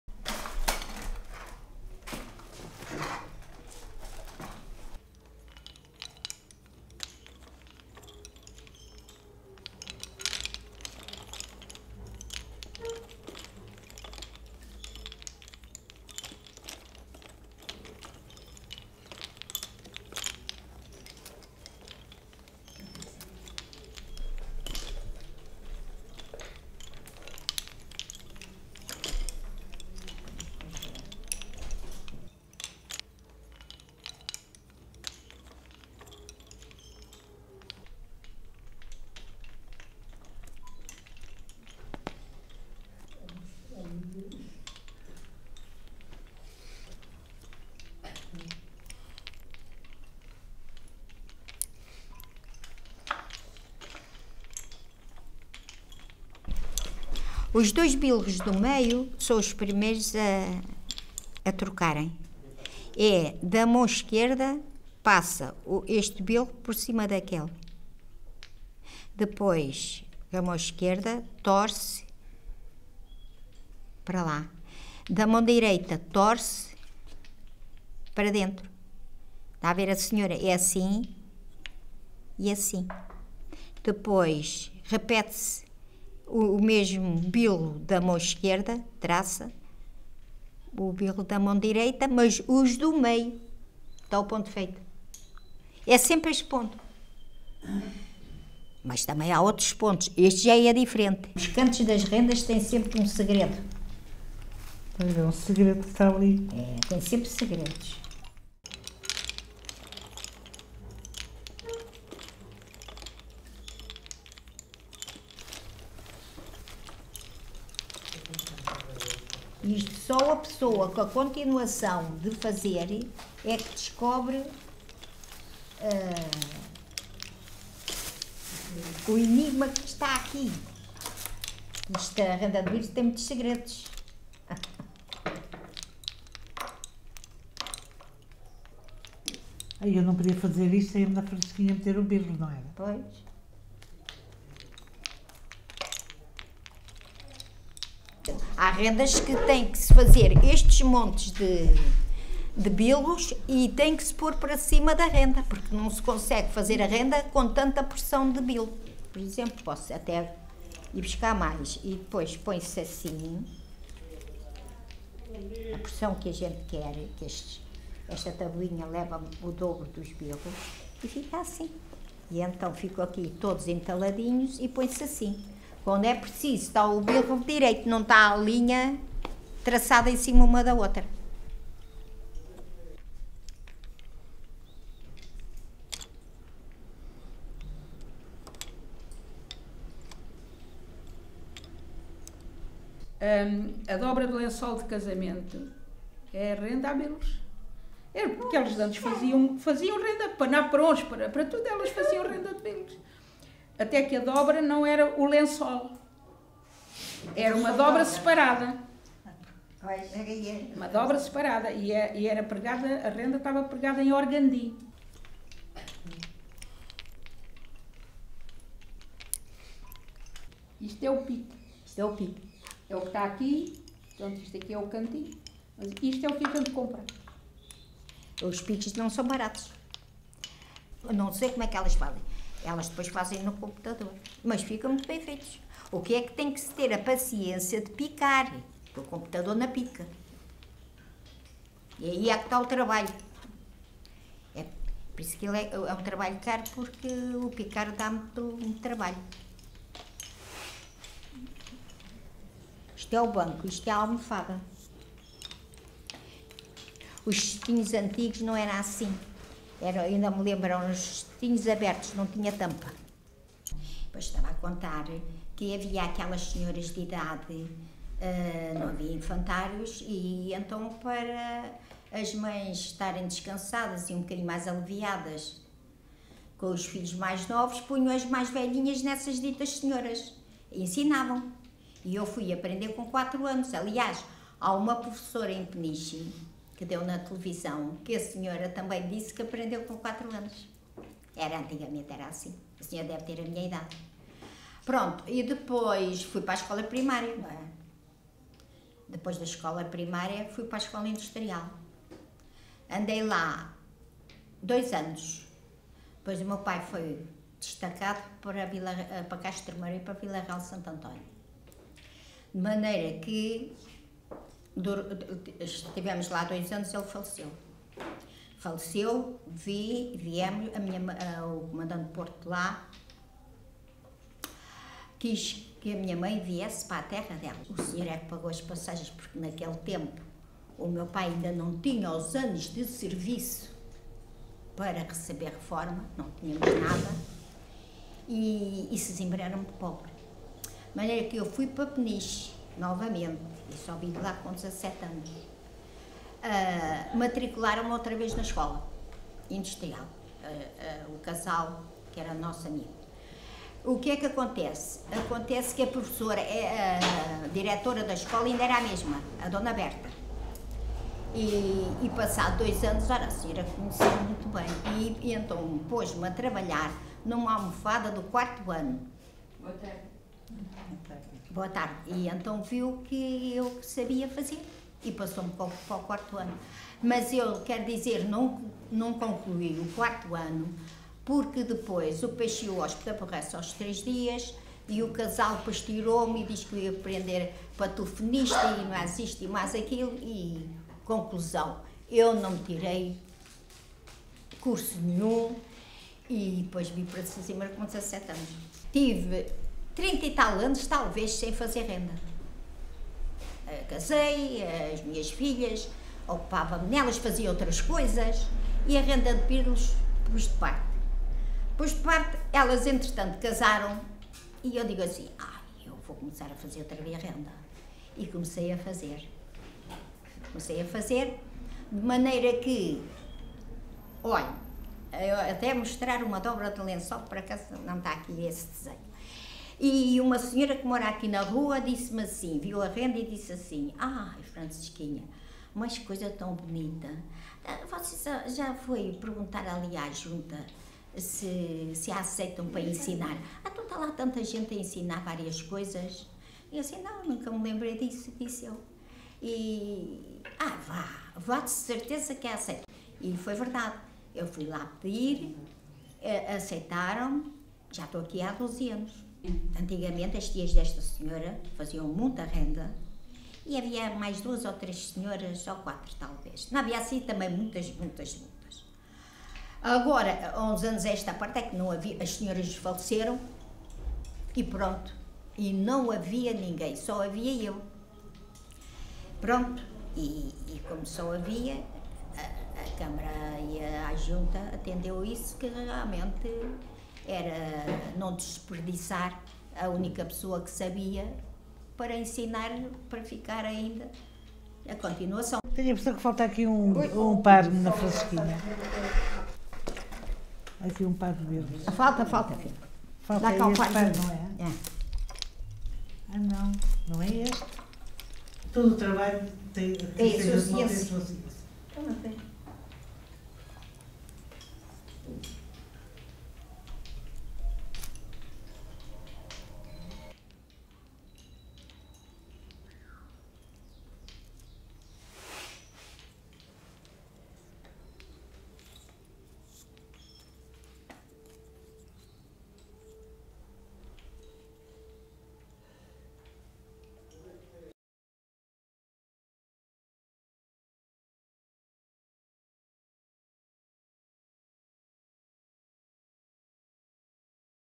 FRANCISCA BATISTA - Mestra de Bilros, Sesimbra
A arte dos Bilros em Sesimbra, pela voz da mestra Francisca Batista. Entrevista: Luís Sousa Martins, João Aldeia, Cláudia Freire.

Realização: José Barbieri

Agradecimento: Cristina Conceição e Município de Sesimbra pelo convite, assistência ao longo das filmagens e integração dos conteúdos produzidos na exposição "Mares de Sesimbra".Sesimbra, 2013